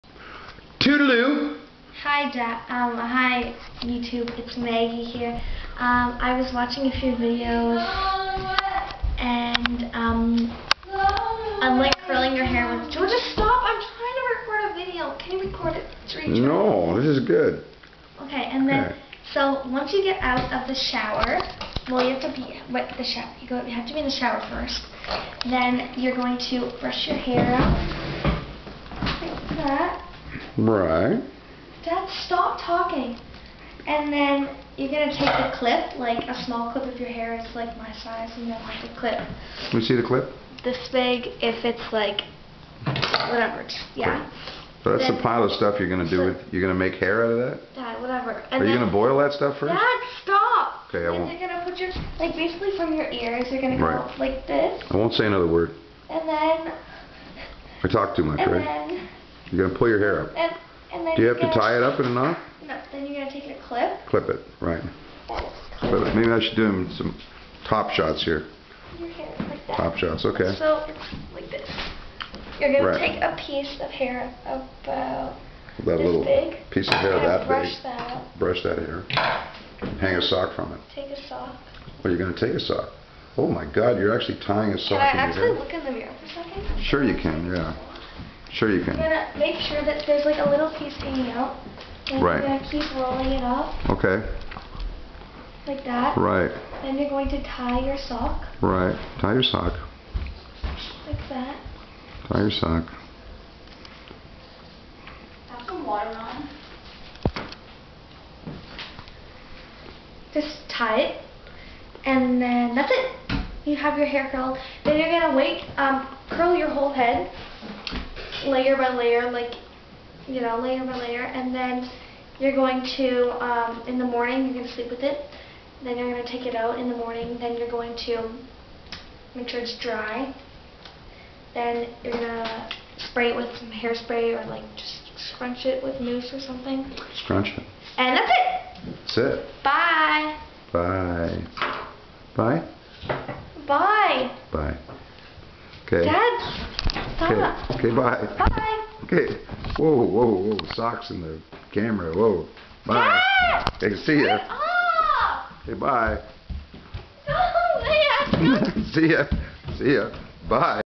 My Old Man Is A Goof! I Hate Him! He's Such A Dork!!!!!!
Hair tip galore from a very insightful young lady.
To bad the old man's a goof.